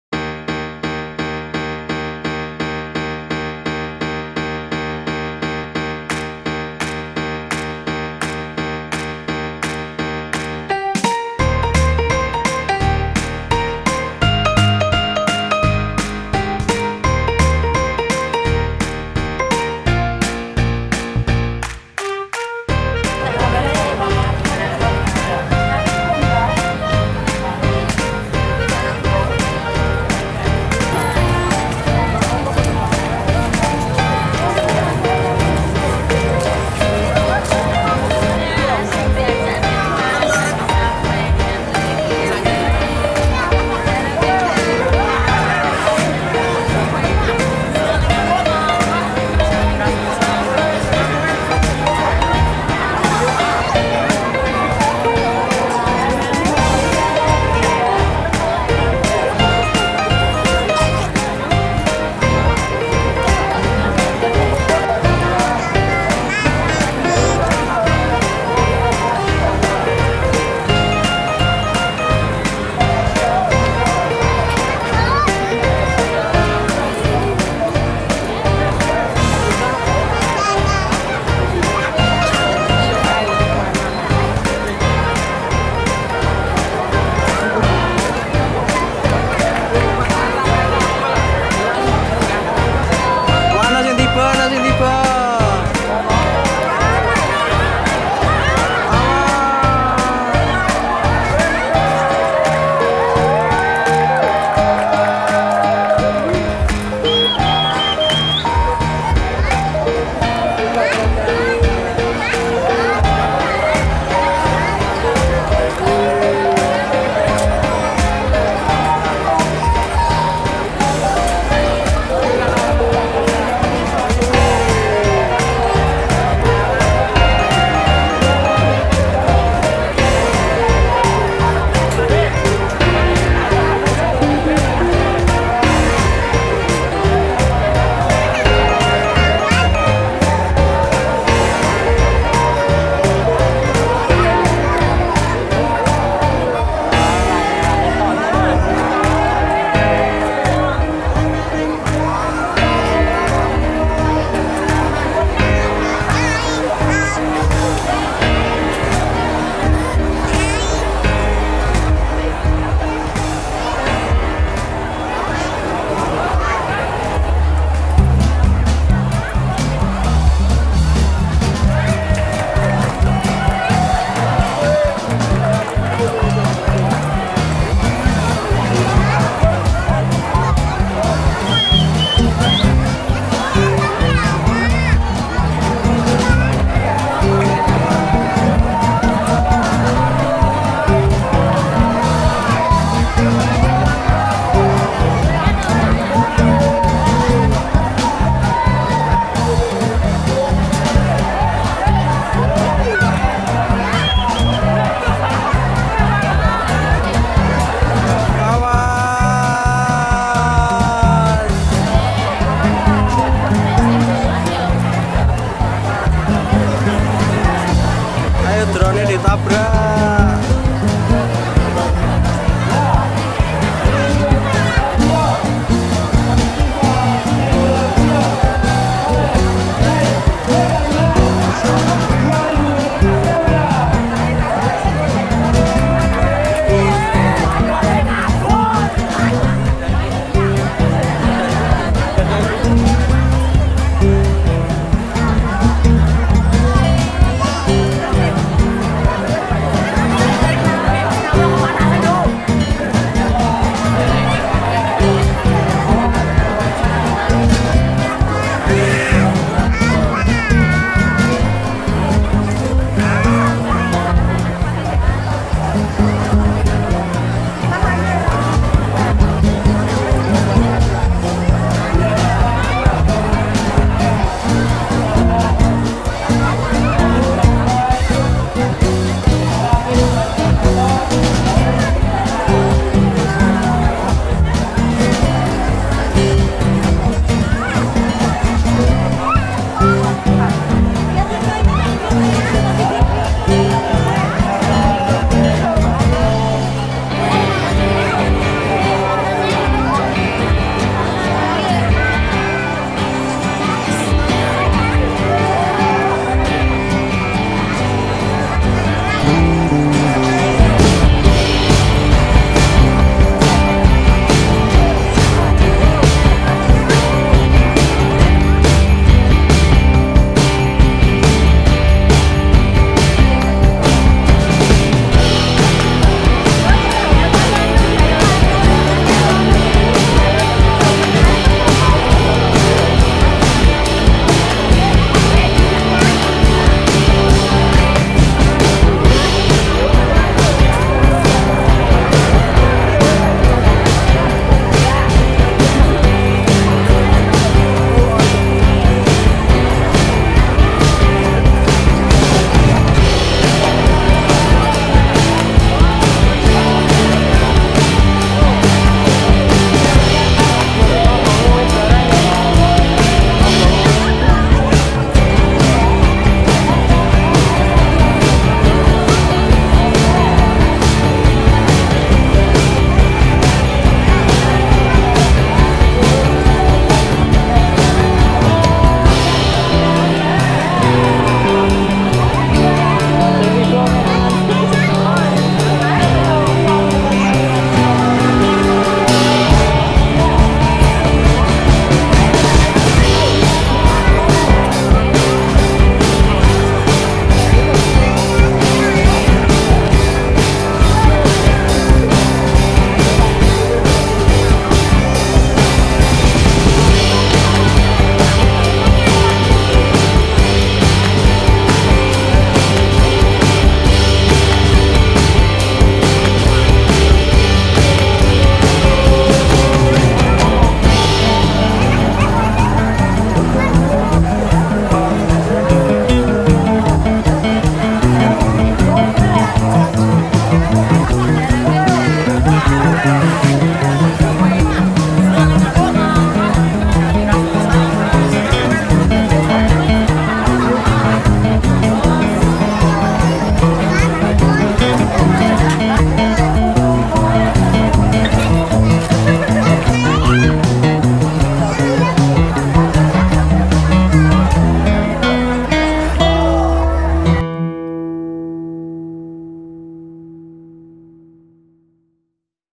1000 Lampion Menoreh Night Spectacular
Langit Perbukitan Menoreh Kulon Progo dihiasi oleh ribuan lampion terbang pada Sabtu 28 Mei 2016. Penerbangan lampion tersebut dikemas dalam acara yang bertajuk Menoreh Night Spectacular (MNS) 2016. Acara diprakarsai oleh Dinas Pariwisata DIY dan Kulon Progo dalam rangka promosi wisata Kulon Progo.